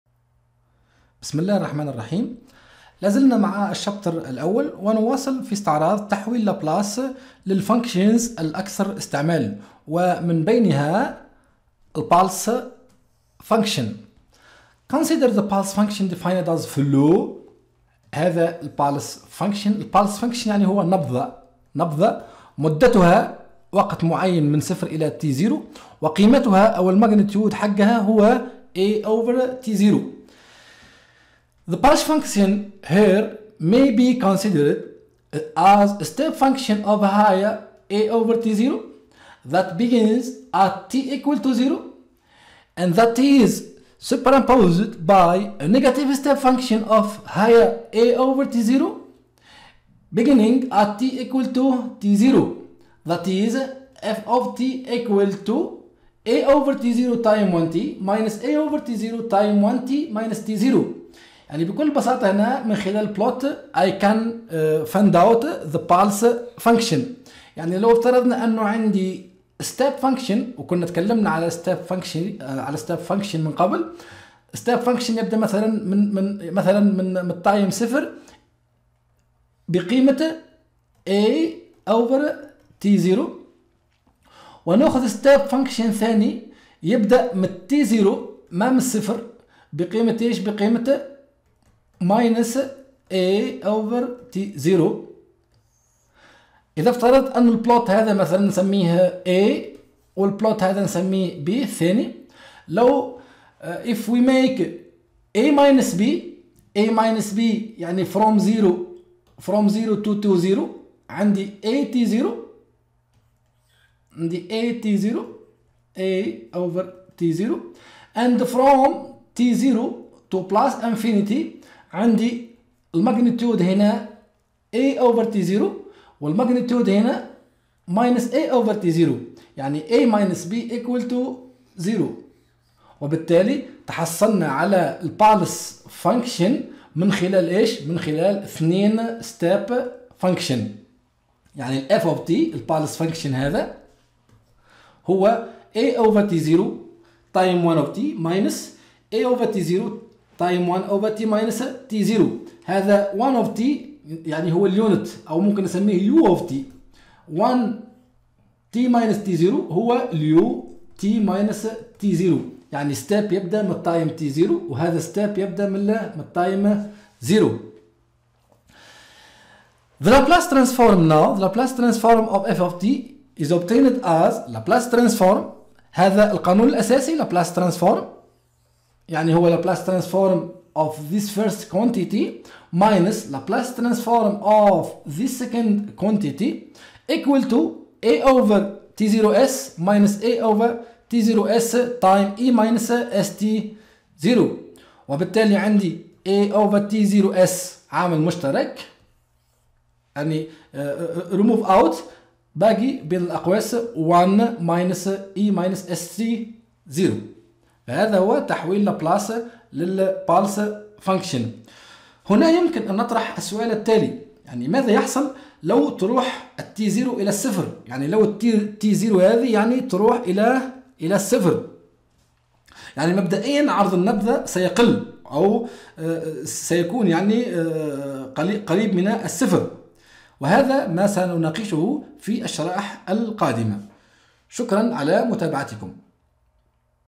Automatic Control | 1-6 | Laplace transform of basics function - 3
الوحدة 1 :  Laplace Transform
الدرس 6 :  Laplace transform of basics function - 3
• Pulse function
• Pulse function from step functions